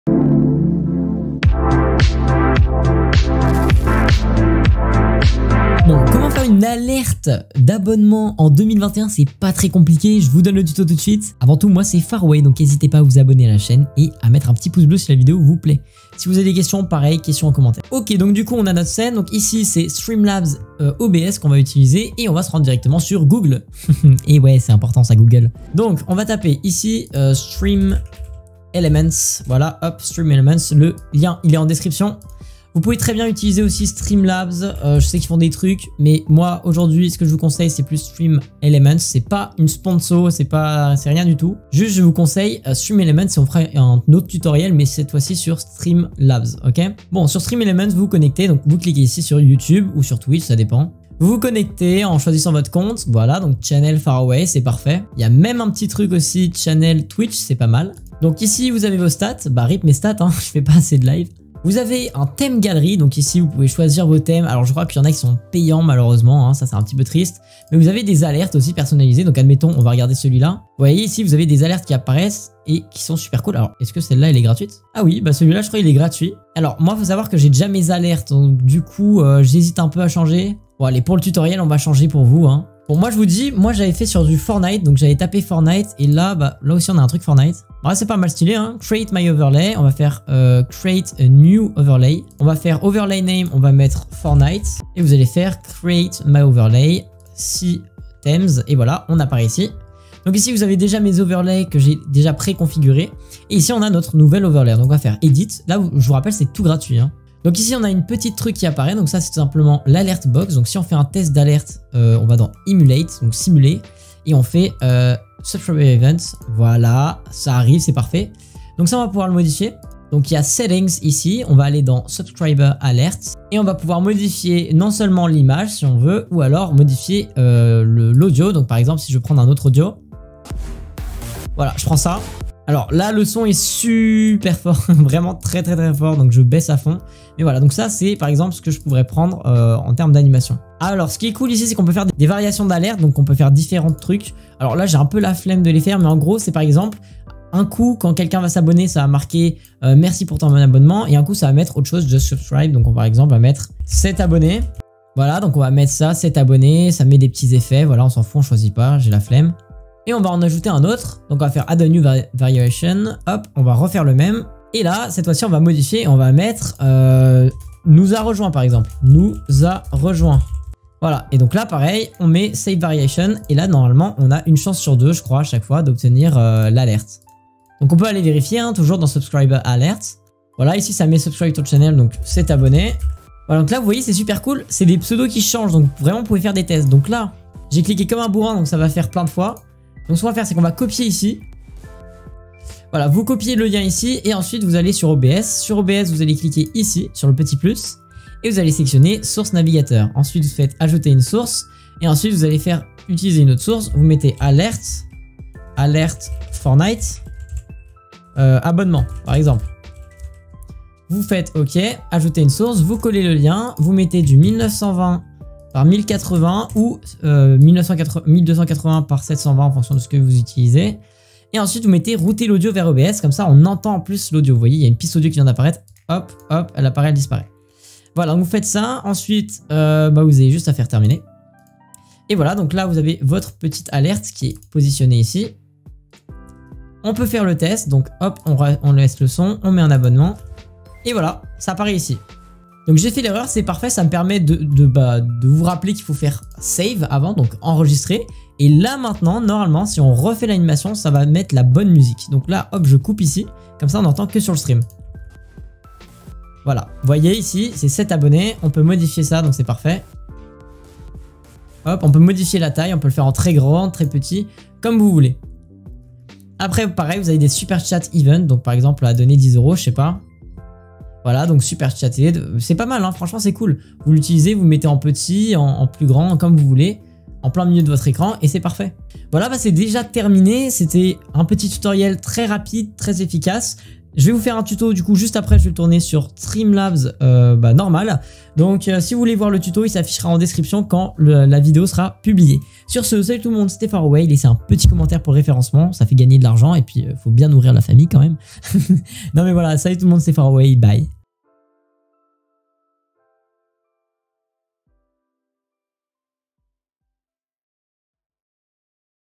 Tuto : Comment faire une MAGNIFIQUE Alerte Abonnement Gratuitement en 2021 [StreamElements]
➤ TUTORIEL : STREAM + CONFIGURER + ALERTES ABONNÉ STREAMLABS OBS / STREAMELEMENTS

👇🏻LIENS IMPORTANTS👇🏻

➤ CODE CRÉATEUR : MYSTIC